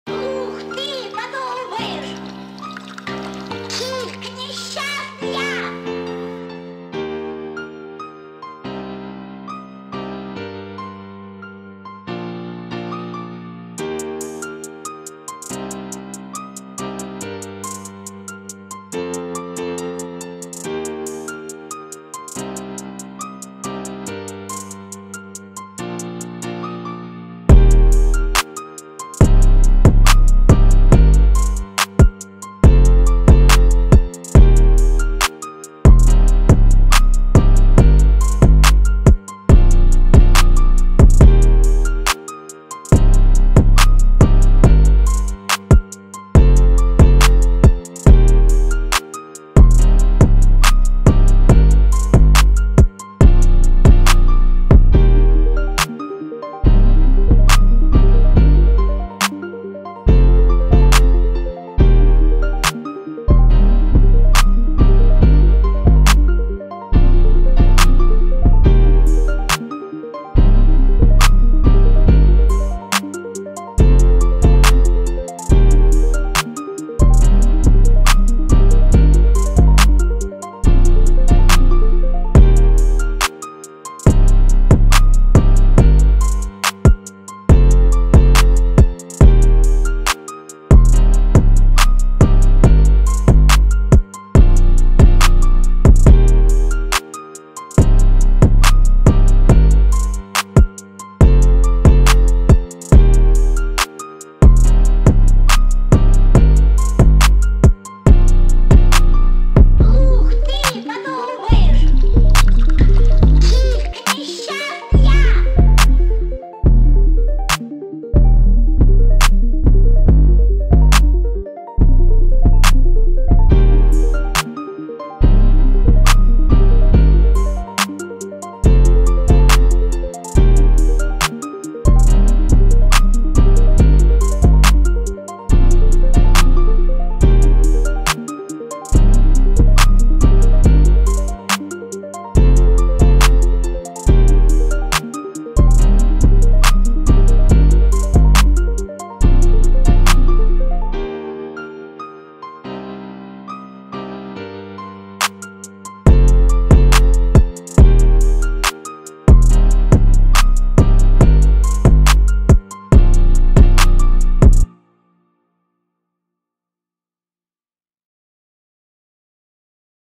⭐free⭐ nick mira x rod wave type beat 2021
𝕓𝕡𝕞: 140
𝕜𝕖𝕪: d# magor

00:00 intro
00:27 chorus
00:54 verse
01:22 chorus
01:49 bridge
01:14 chorus
02:03 verse
02:30 outro

(𝕛𝕦𝕤𝕥 𝕥𝕒𝕘𝕤)